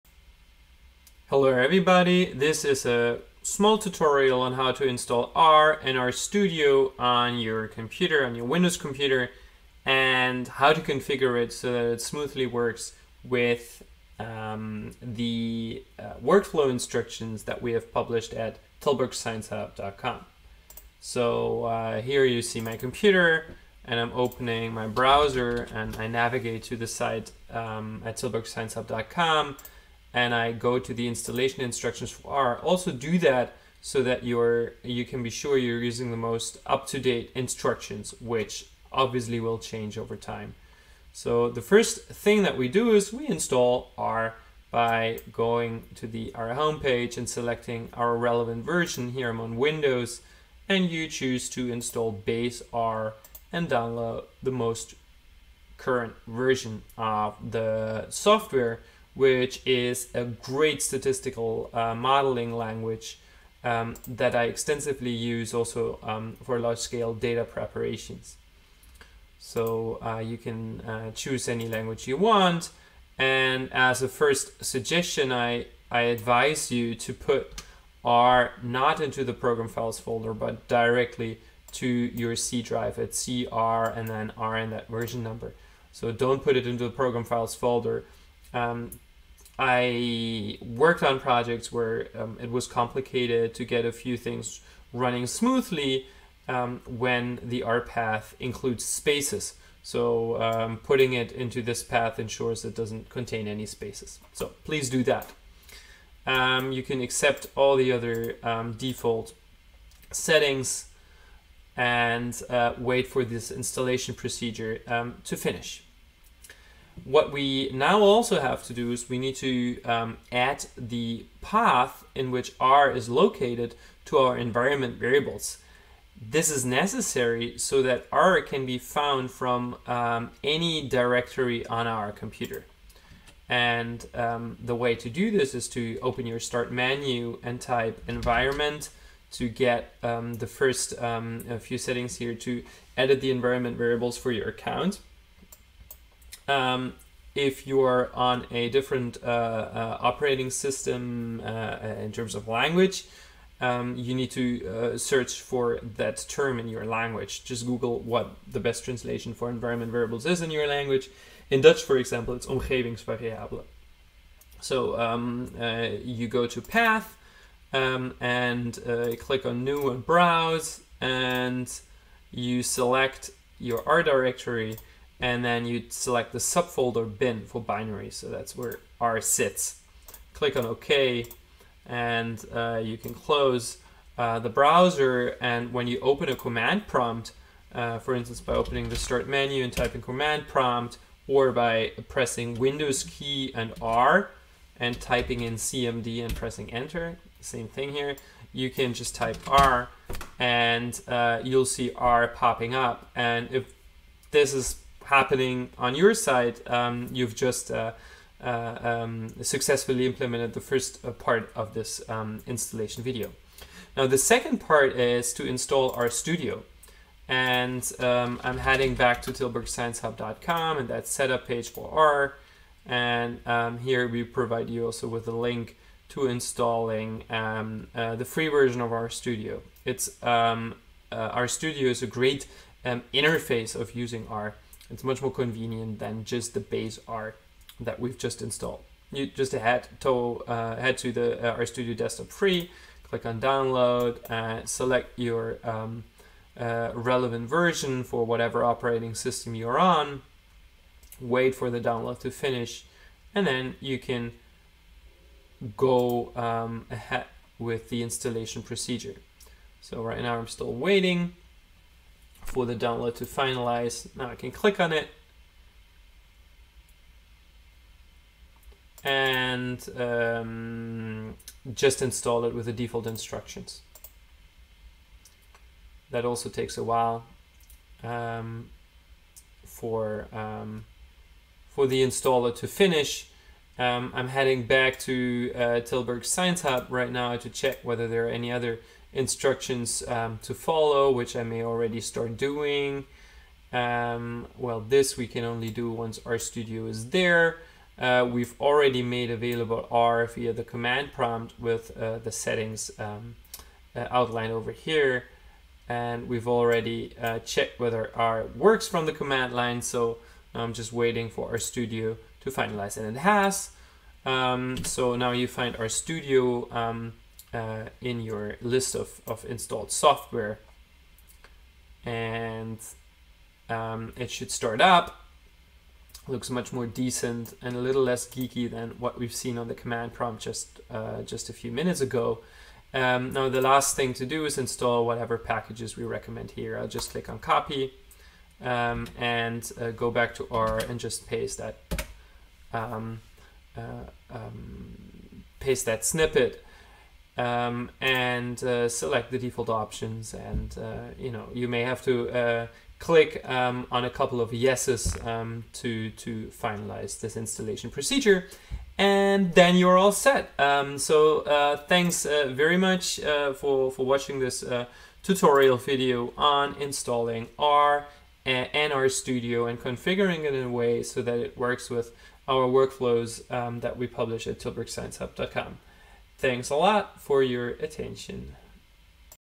Installing R and RStudio on Windows - tilburgsciencehub.com
In this video, I walk you through the setup of R and RStudio and show you how to configure R to use it for managing data- and computation-intensive projects. I pay specific attention to adding R to the system's environment variables (so that you can use it from anywhere on your computer).